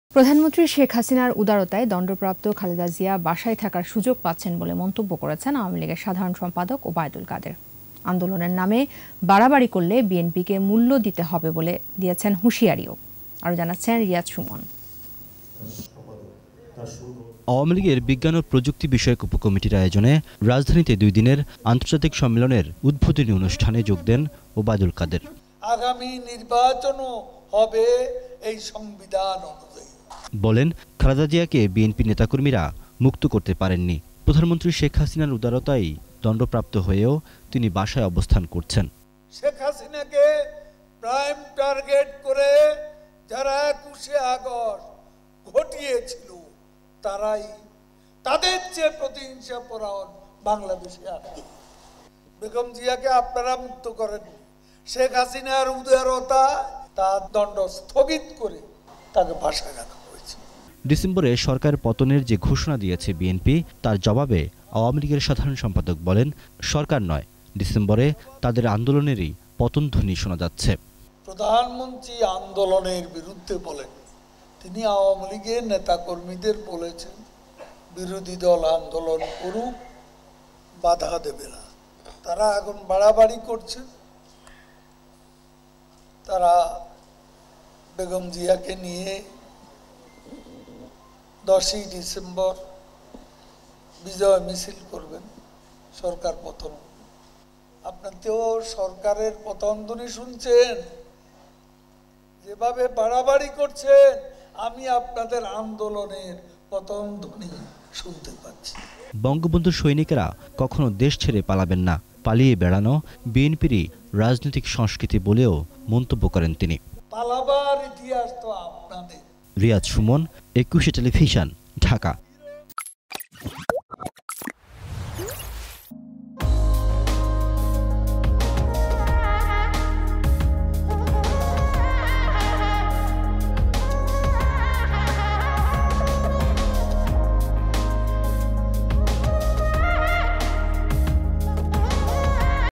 শেখ হাসিনার উদারতায়ই দণ্ডপ্রাপ্ত খালেদা জিয়া বাসায় থাকার সুযোগ পাচ্ছেন || ETV News
প্রধানমন্ত্রী শেখ হাসিনার উদারতায়ই দণ্ডপ্রাপ্ত খালেদা জিয়া বাসায় থাকার সুযোগ পাচ্ছেন বলে মন্তব্য করেছেন আওয়ামী লীগের সাধারণ সম্পাদক ওবায়দুল কাদের। আন্দোলনের নামে বাড়াবাড়ি করলে বিএনপিকে মূল্য দিতে হবে বলে দিয়েছেন হুশিয়ারিও। আরো জানাচ্ছেন রিয়াজ সুমন।